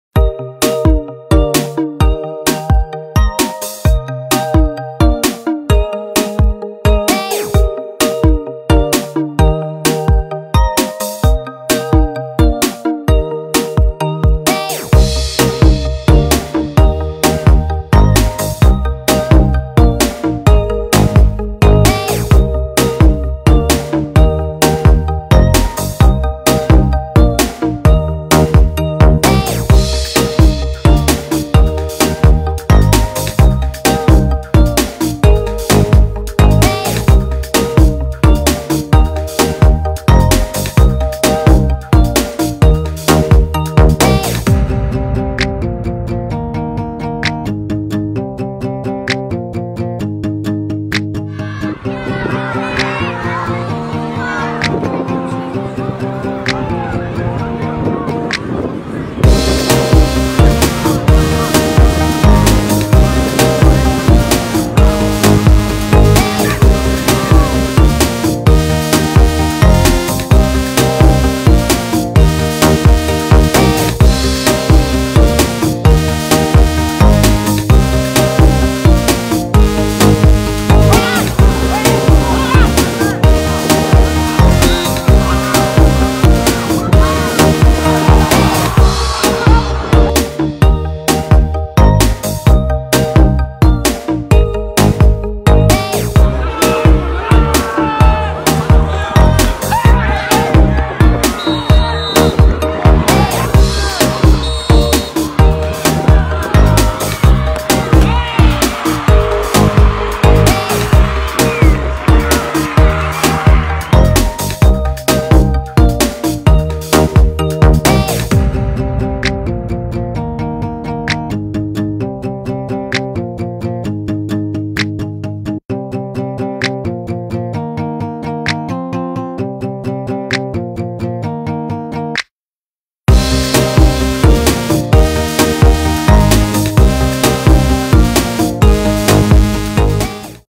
Mecz reprezentacji polski na stadionie OSiR we Włocławku. Polska - Grecja 2:1
27 marca na stadionie Ośrodka Sportu i Rekreacji we Włocławku reprezentacja Polski zwyciężyła 2-1 z Grecją. Do przerwy Polska prowadziła 1-0.